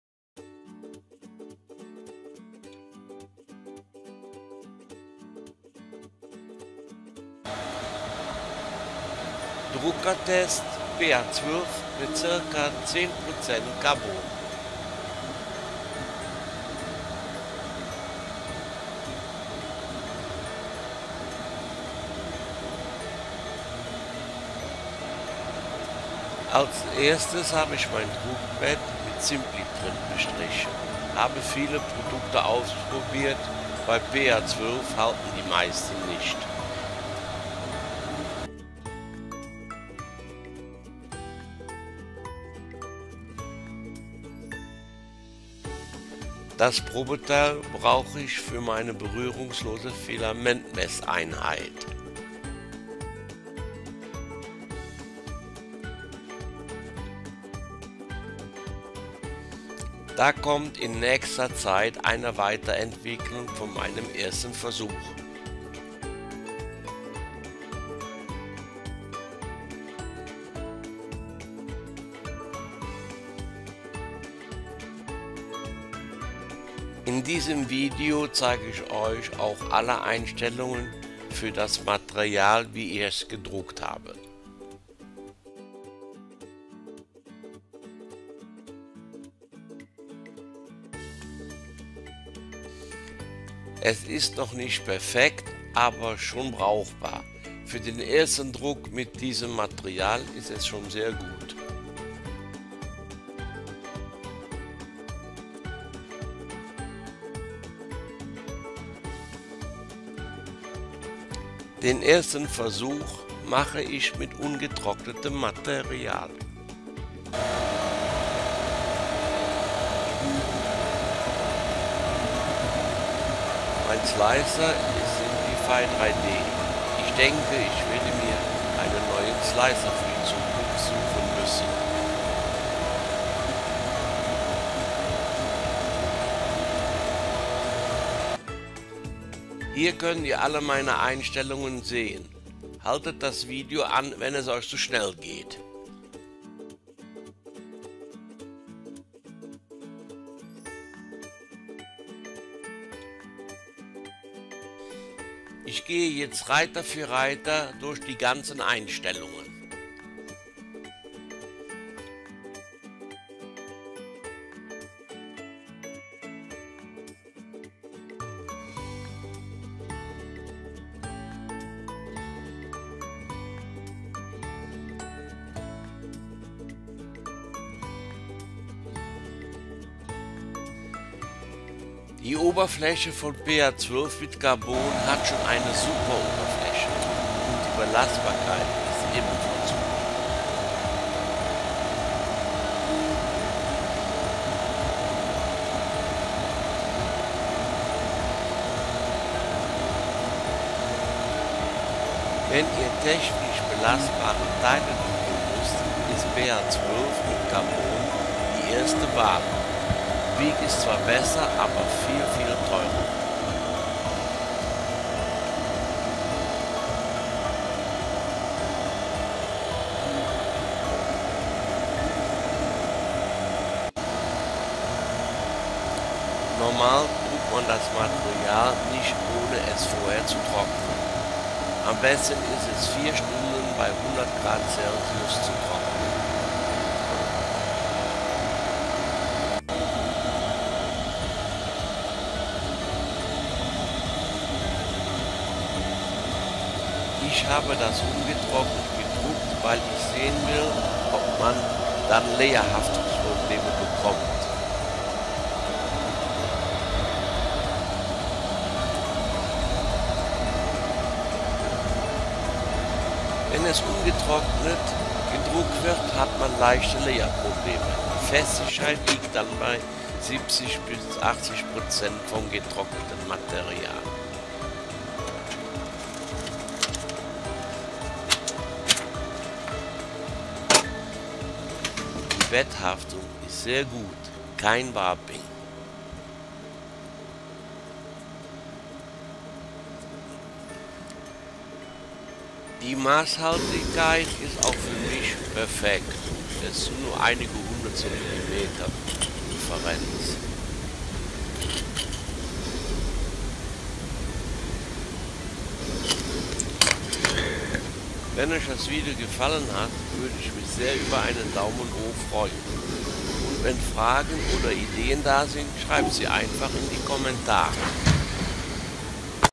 Filament Print Test PA12Carbon from Extruded Material
Today we want to test the last week extruded PA12 with 10% carbon whether it is also good to print on the printer. I take the material as it came out of the extruder. Not dried that has the disadvantage that the surface is not so nice the layer adhesion is not so good and some other disadvantages. But as you can see in my video, it still prints well without warping. Now you can imagine that it dries much better and the surface will be perfect. In one of my next videos I will test the material then completely.
Filament Drucktest PA12Carbon vom Extrudierten Material
Heute wollen wir das letzte Woche extrudierte PA12 mit 10 % Carbon testen ob es auf dem Drucker auch gut zu Drucken ist. Das Material nehme ich so wie es aus dem Extruder raus gekommen ist. Nicht getrocknet das hat den Nachteil das die Oberfläche nicht so schön ist die Layerhaftung ist nicht so gut und noch einige andere Nachteile. Aber wie ihr in meinen Video sehen könnt läßt es sich trotzdem gut Drucken ohne Warping. Jetzt könnt ihr euch ja Vorstellen das es getrocknet sich viel besser verarbeiten läßt und die Oberfläche dann perfekt wird. In einer meiner nächsten Videos werde ich das Material dann komplett testen.